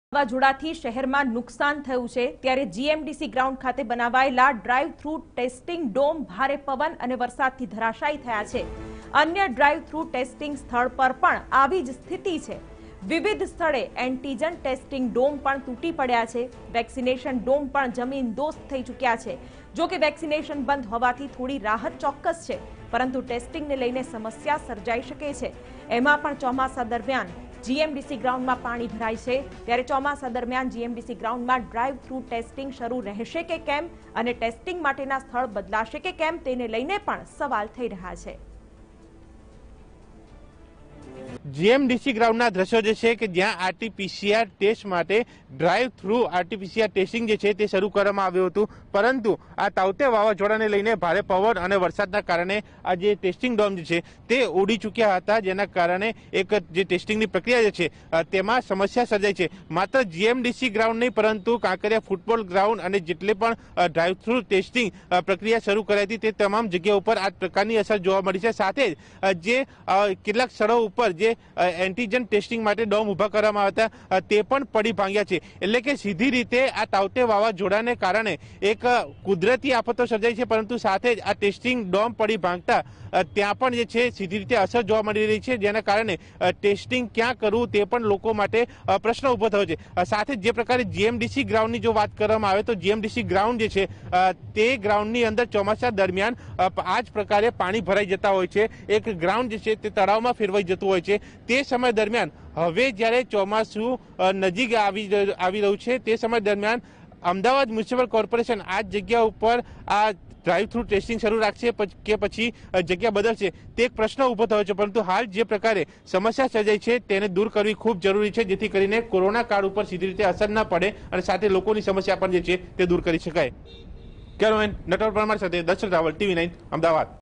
Ahmedabad: Many vaccination and antigen testing domes blown off following Cyclone Tauktae| TV9News
Ahmedabad: Many vaccination and antigen testing domes blown off following Cyclone Tauktae| N24